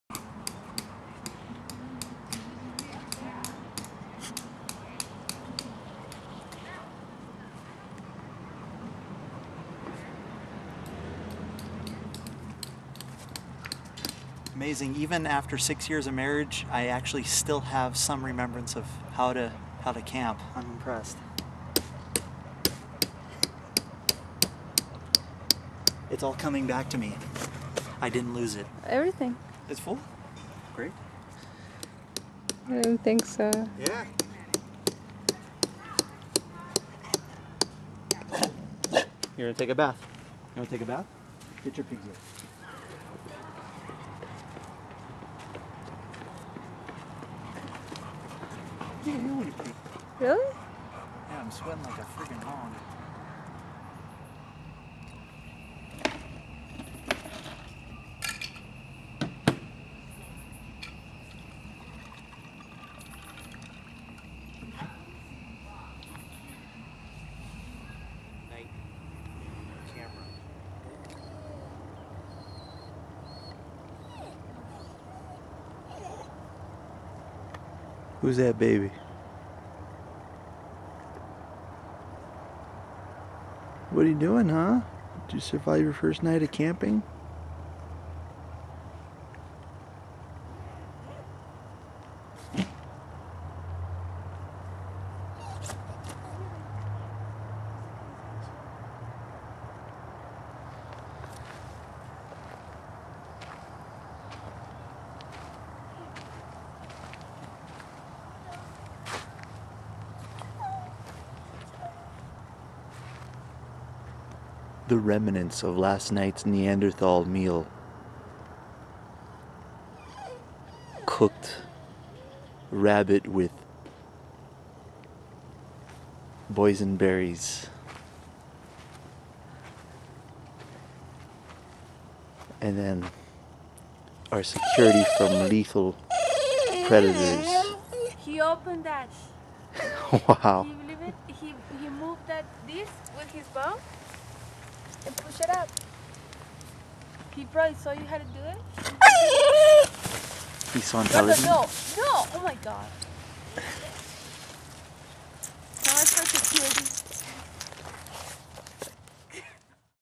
English Bulldog Camping in Tent
Our English Bulldog really did not like "roughing-it" and would have rather slept on our king size for sure!

This was a fun first camping trip for him on the North side of Big Bear in Southern, California.